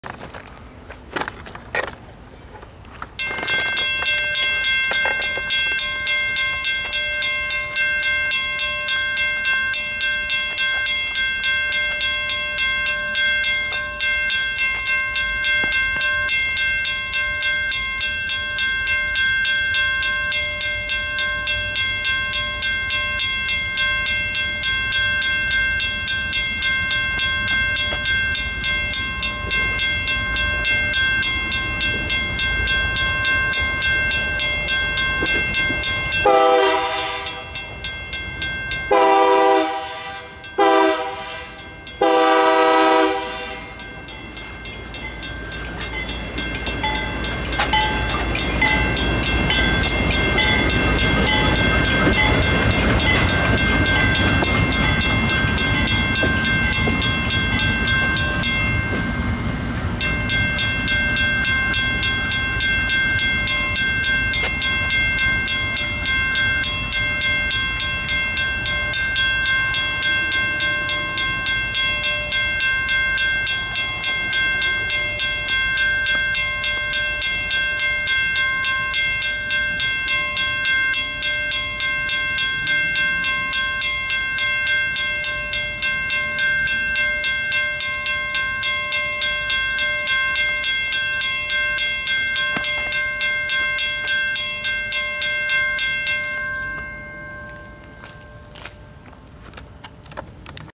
Meadowbrook Drive Railroad Crossing, Huntsville, AL
Signal on the left: 2 pairs of Modern Industries 12x20 inch lights.
Signal on the right: 2 pairs of Modern Industries 12x20 inch lights and a WCH mechanical bell.

This was the last crossing I decided to record the HMCR at today.